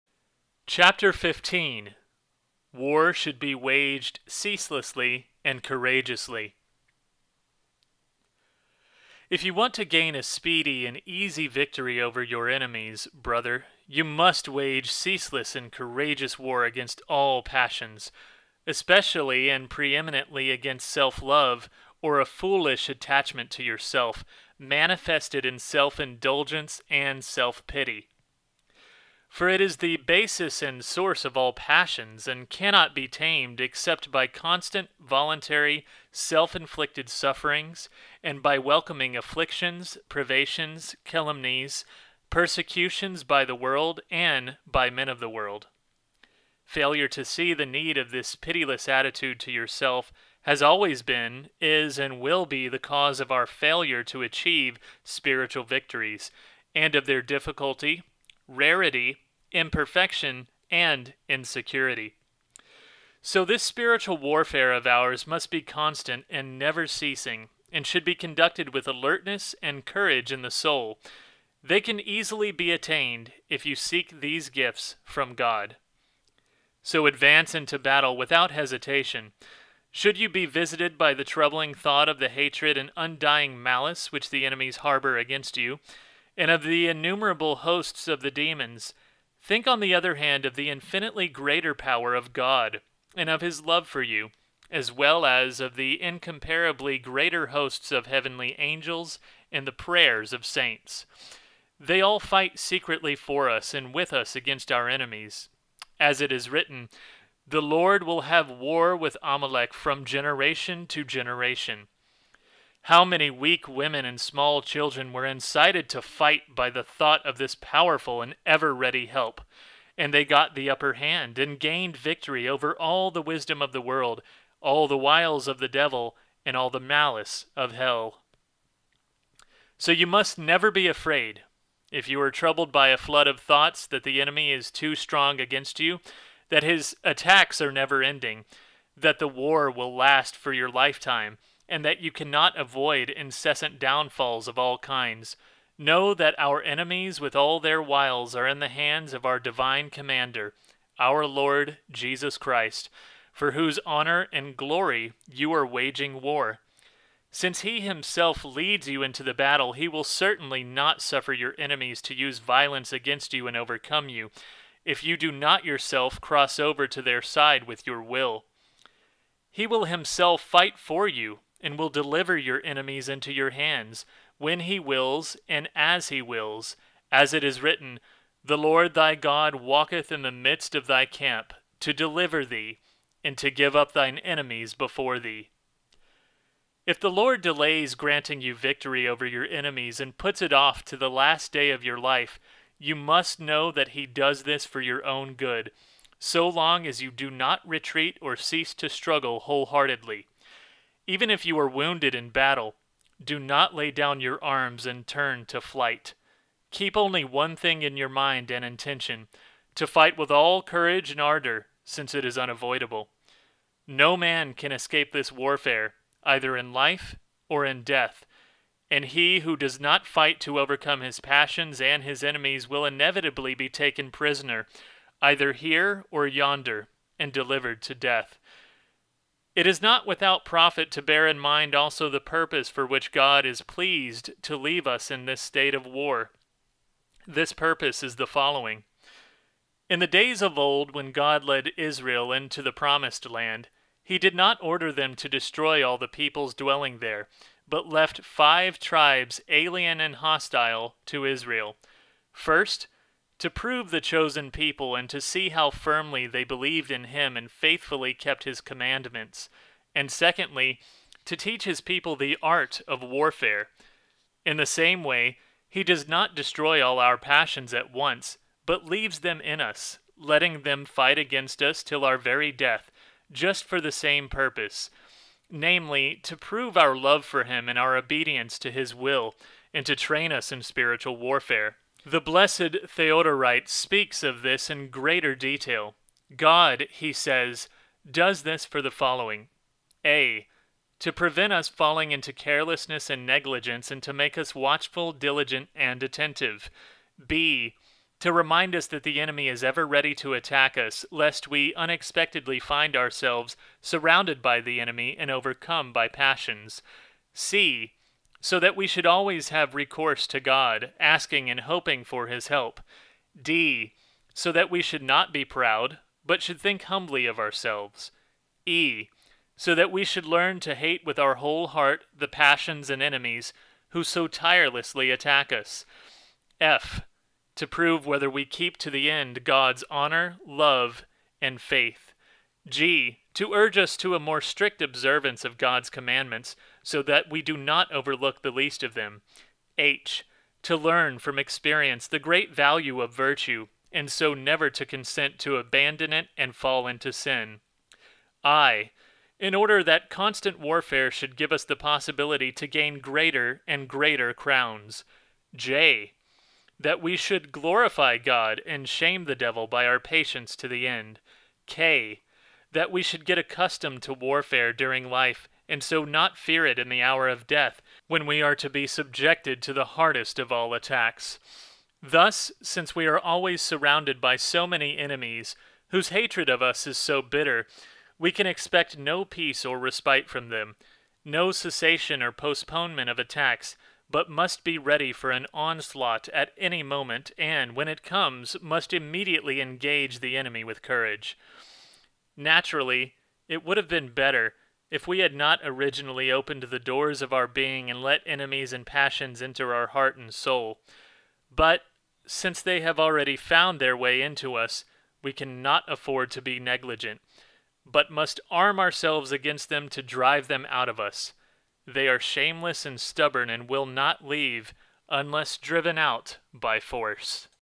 Unseen Warfare, Part One, Chapter Fifteen: War Should Be Ceaseless and Courageous
This is the fifteenth chapter of the book 'Unseen Warfare' as edited by St. Nicodemus of the Holy Mountain and revised by St. Theophan the Recluse.

War should be waged ceaselessly and courageously